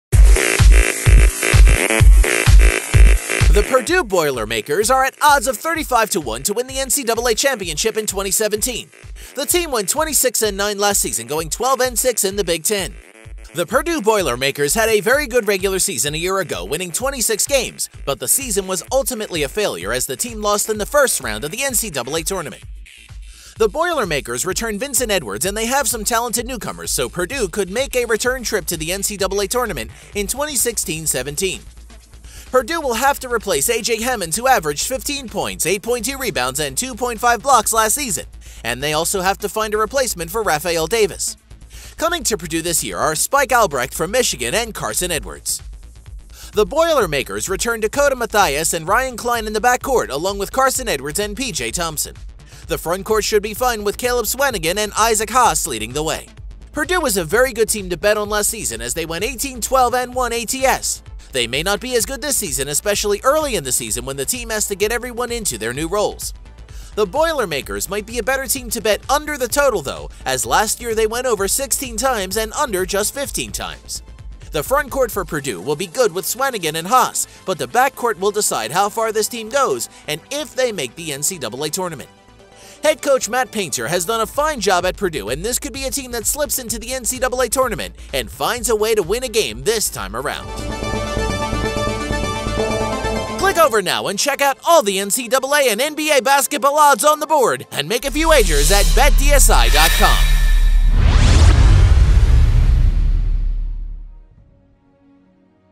Purdue Boilermakers NCAA Basketball 2016 17 Team Preview Predictions & Betting Odds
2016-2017 NCAA Championship Odds: 35-1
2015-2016 Record: 26-9, 12-6 Big Ten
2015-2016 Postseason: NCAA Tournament

 The  Purdue Boilermakers  are at odds of 35-1  to win the NCAA Championship in 2017.  The team went  26-9,, last season,  going 12-6 in the  Big Ten
The Purdue Boilermakers had a very good regular season a year ago winning 26 games but the season was ultimately a failure as the team lost in the first round of the NCAA Tournament.
The Boilermakers return Vincent Edwards and they have some talented newcomers so Purdue could make a return trip to the NCAA Tournament in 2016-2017.

Purdue will have to replace A.J. Hammons who averaged 15 points, 8.2 rebounds and 2.5 blocks last season and they also have to find a replacement for Raphael Davis. Coming to Purdue this year are Spike Albrecht from Michigan and Carsen Edwards.
The Boilermakers return Dakota Mathias and Ryan Cline in the backcourt along with Carsen Edwards and P.J. Thompson. The frontcourt should be fine with Caleb Swanigan and Isaac Haas leading the way.